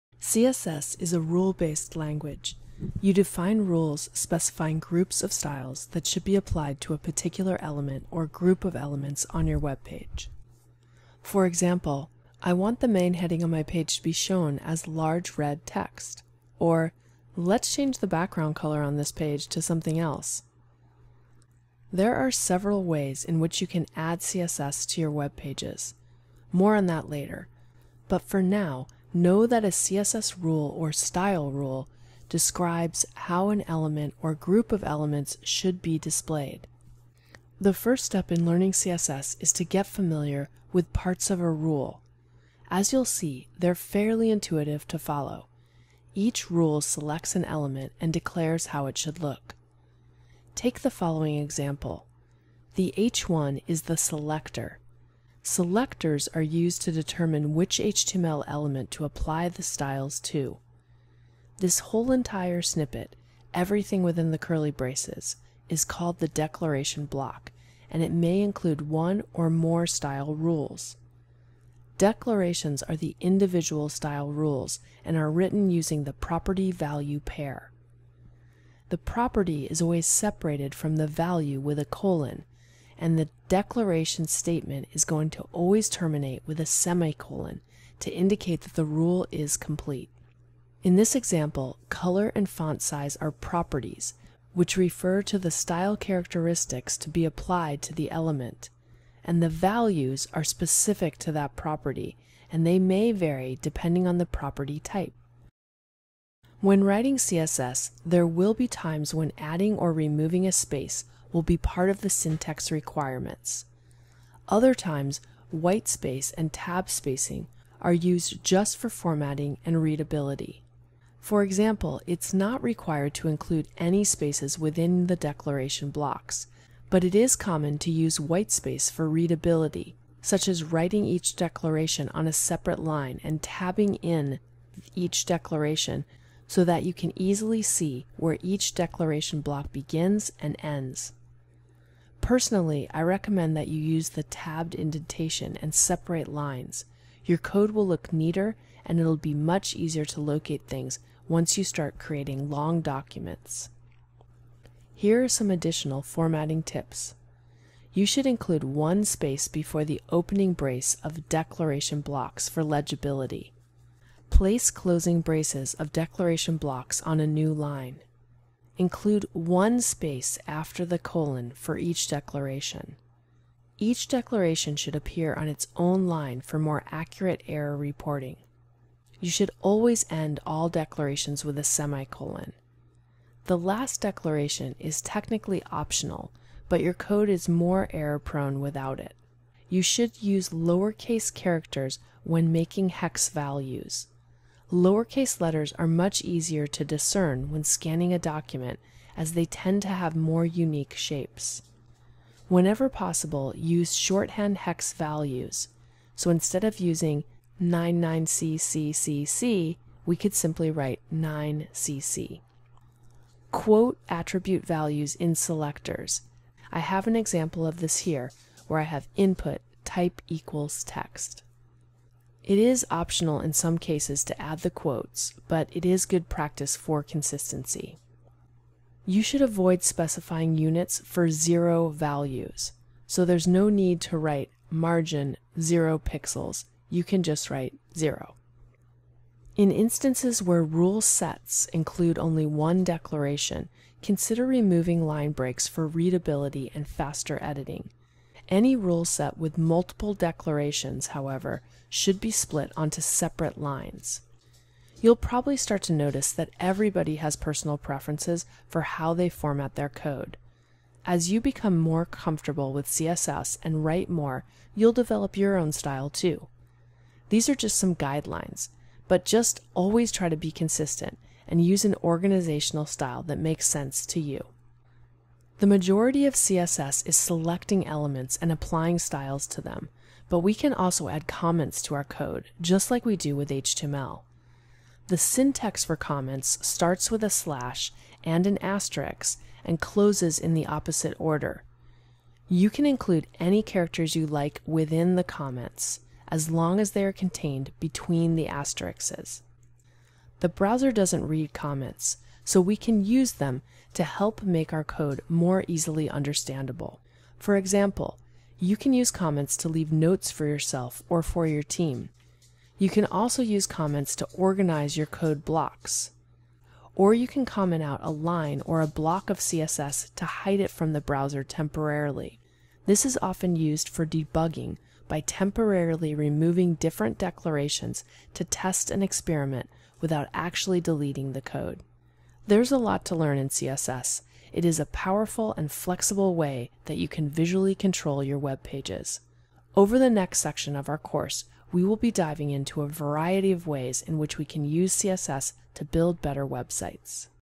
CSS syntax
In order to start using CSS you should be familiar with the syntax and terminology. In addition knowing the guidelines and rules of what makes CSS code valid and function correctly is paramount. Here I will give you guidelines for best practices in conjunction with CSS and show you how to write CSS comments.